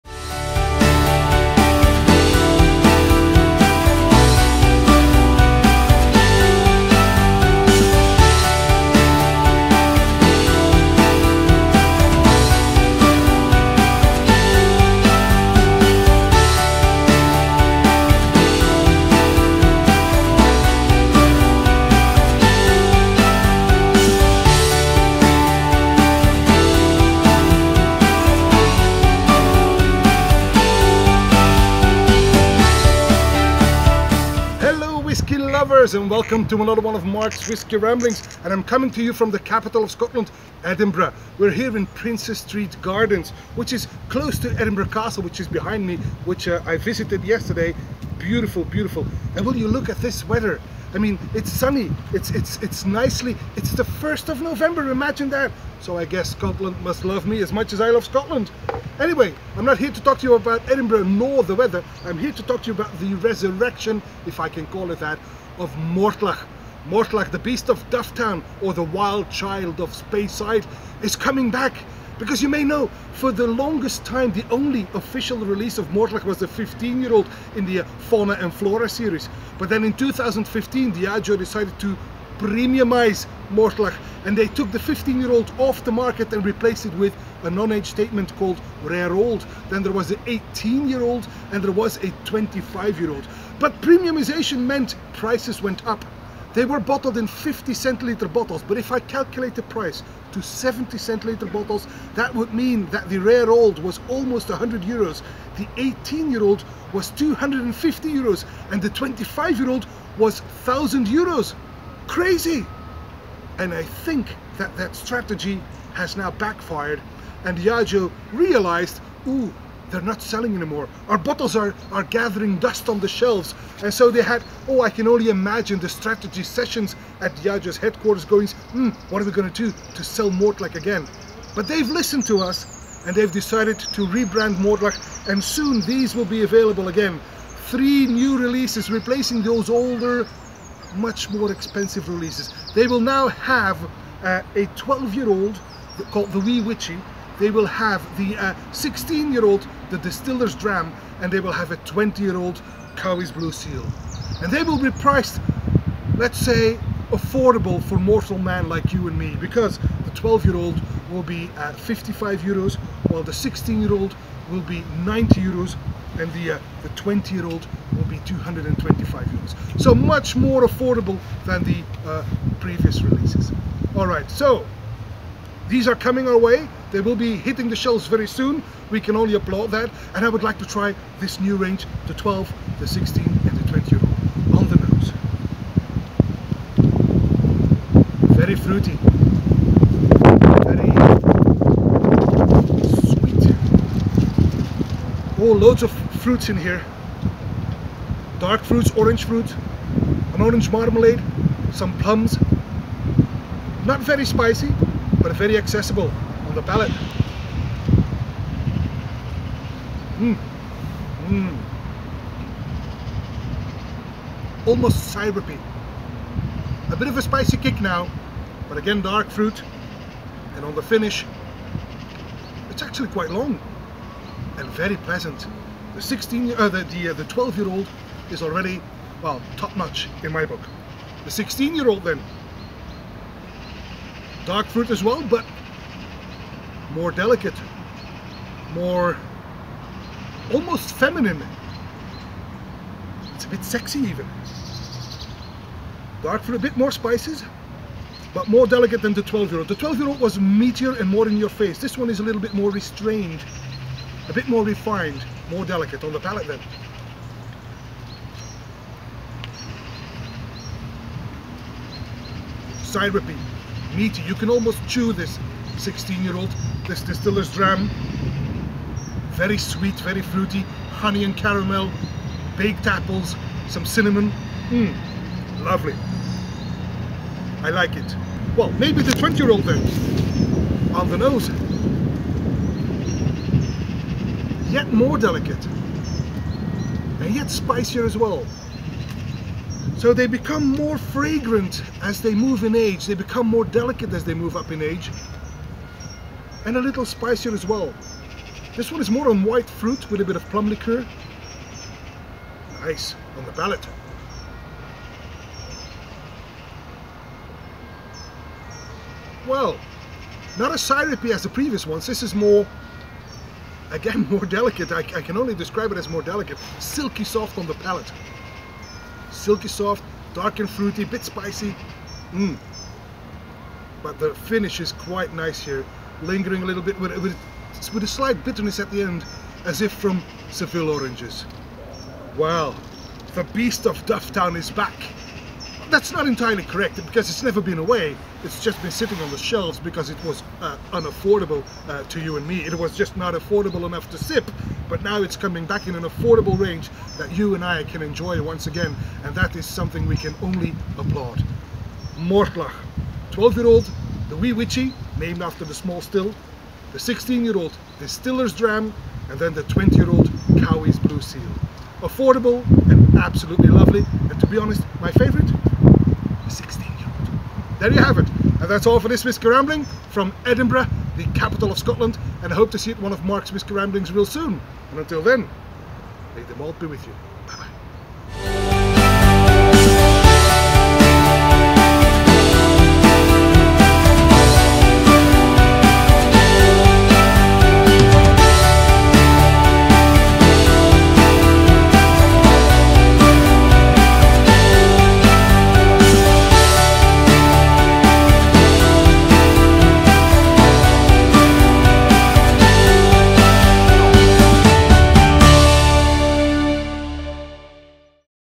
Mark’s Whisky Ramblings 223: Mortlach’s new range – 12, 16, 20 Year Old
Mark Dermul, Belgian whiskyblogger, sheds his light on the revamped Mortlach Range. For the longest time, Mortlach 16 Year Old in the Fauna & Flora series was the only one available. But in 2015 it was replaced by a new range in 50cl bottles… at prohibitive prices. The result: the bottles gathered dust on the shelves. Diageo had to rethink their Beast of Dufftown strategy and came up with a new range with a price tag that mortal man can again afford. Hurray! Let’s have a look at those three new releases!